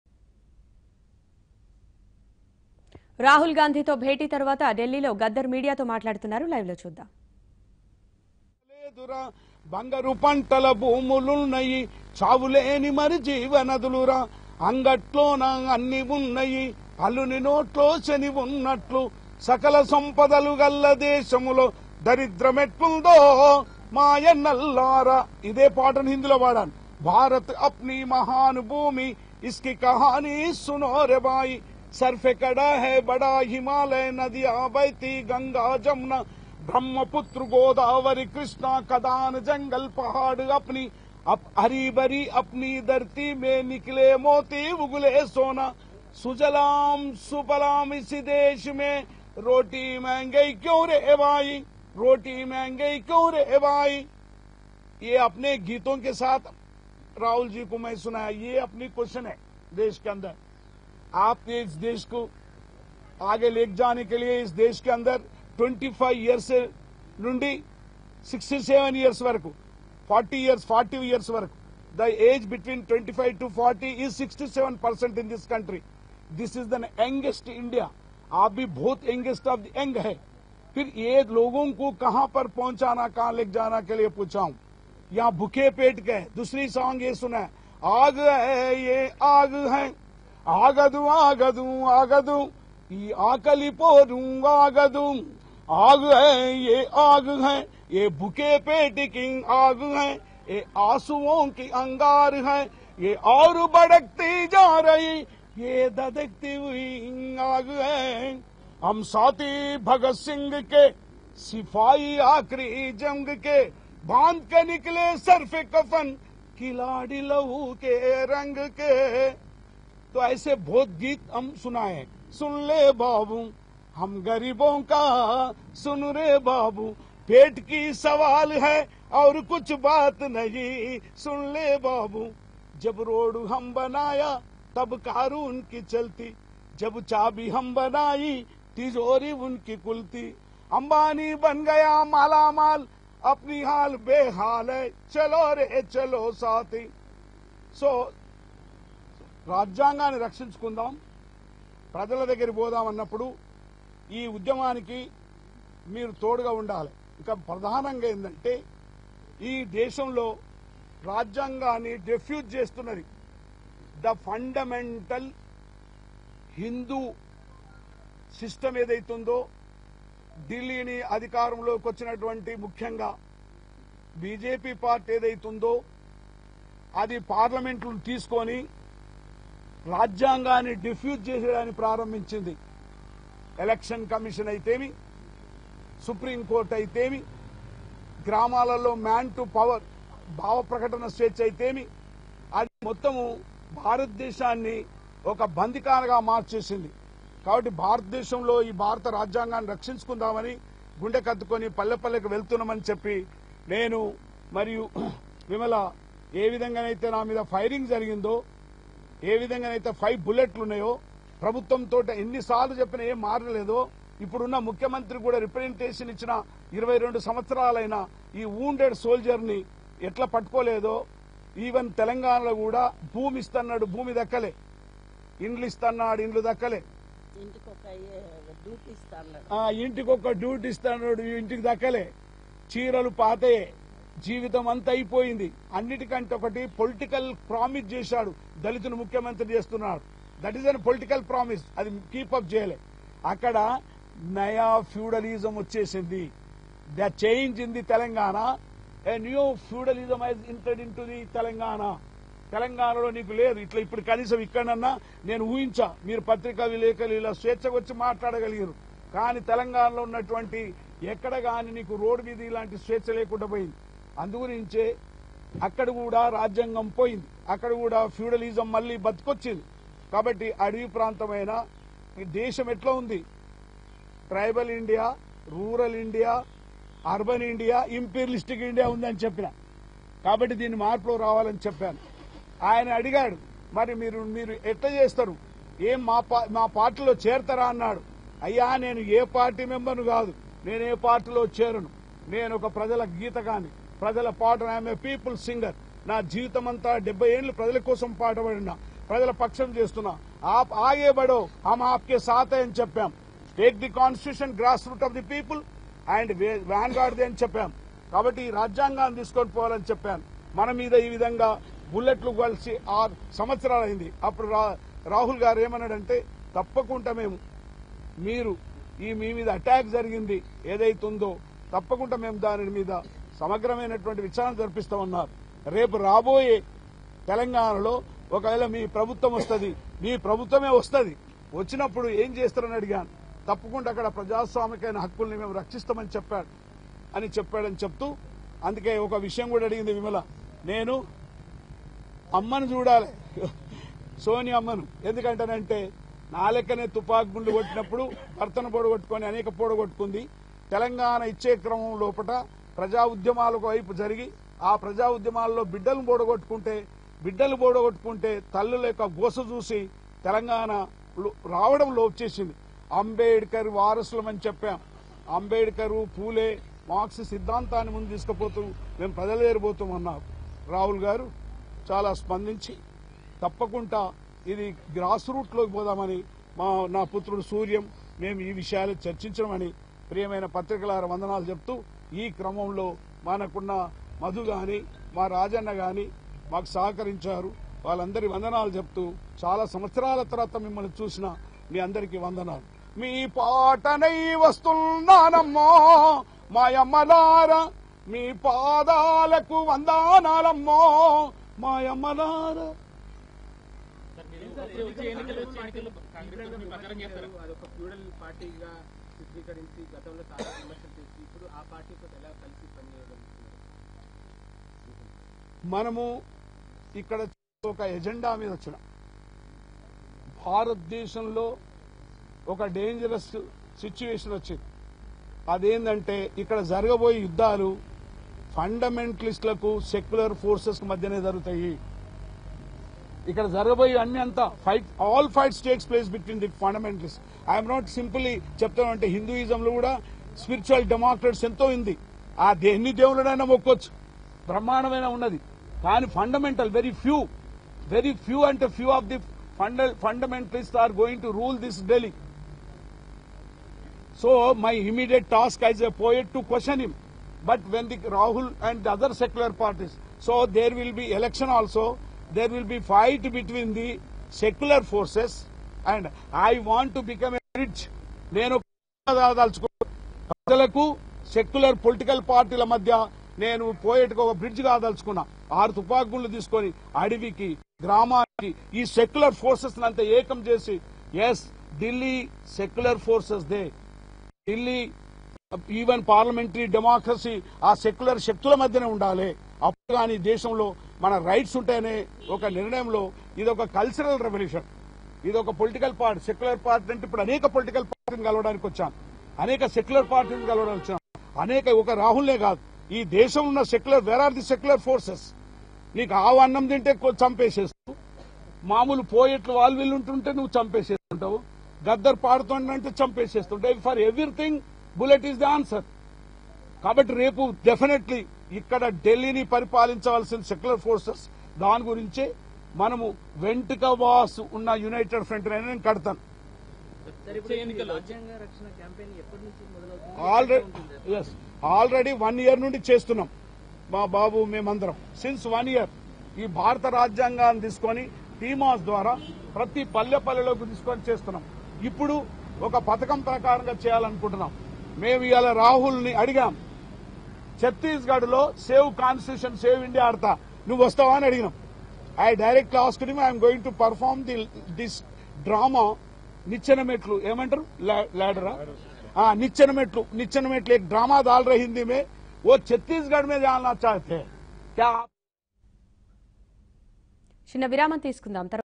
Gaddar speaks to media after meeting with Rahul Gandhi | ABN Telugu
Gaddar speaks to media after meeting with Congress president Rahul Gandhi saying that he spoke to him in the form of song and about KCRs feudalism. He also clarifies that not joining the party because he is just a people's singer.